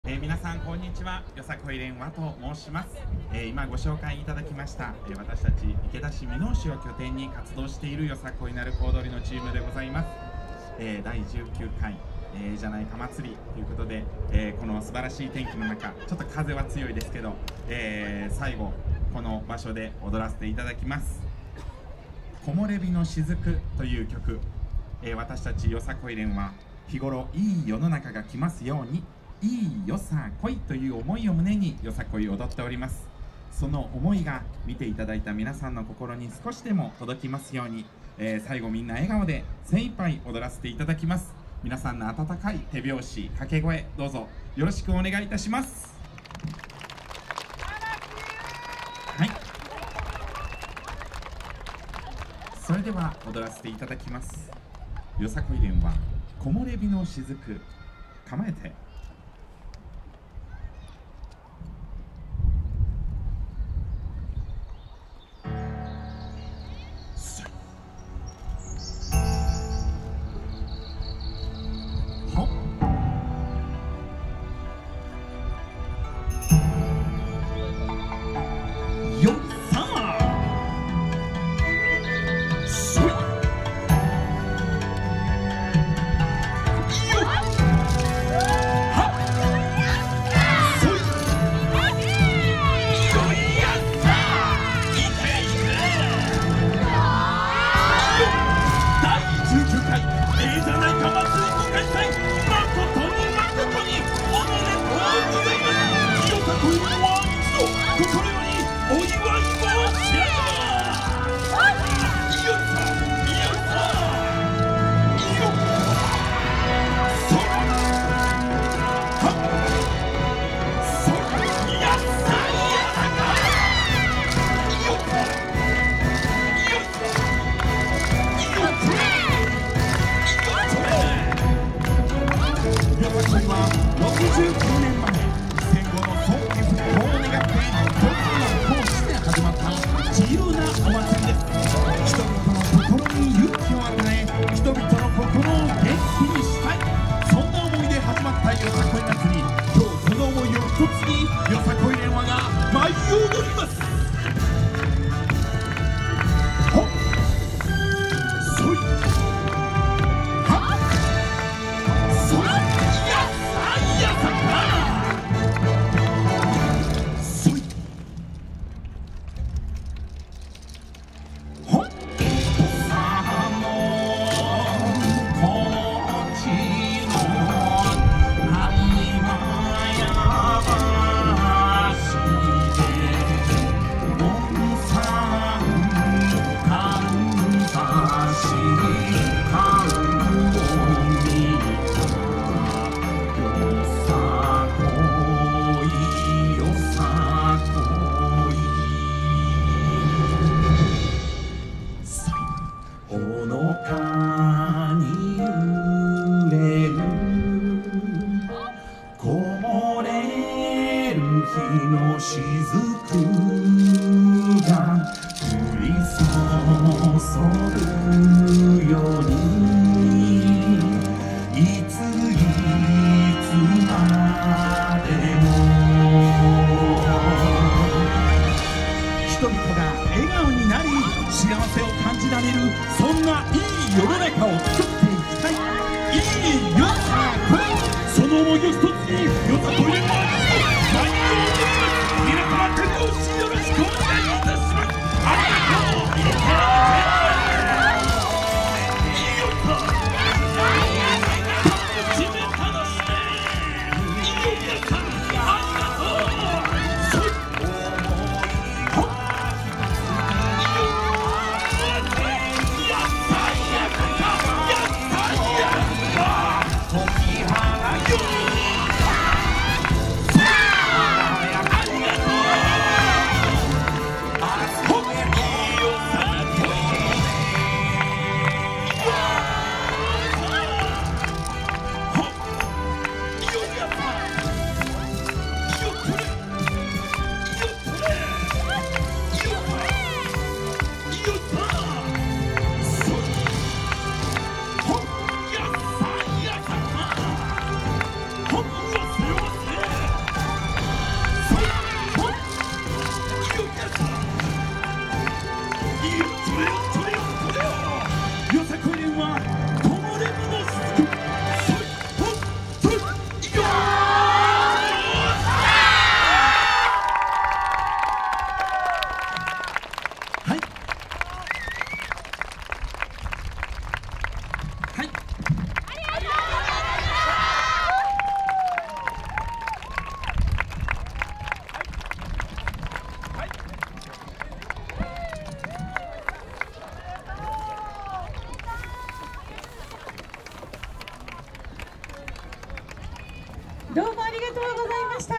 よさこい連「わ」 📍泉州よさこい ゑぇじゃないか祭り (石舞台会場) 2023-10-15T15:57
2023年10月15日(日)、大阪府泉佐野市で開催された「泉州よさこい ゑぇじゃないか祭り」(石舞台会場)での「よさこい連「わ」」さんの演舞です。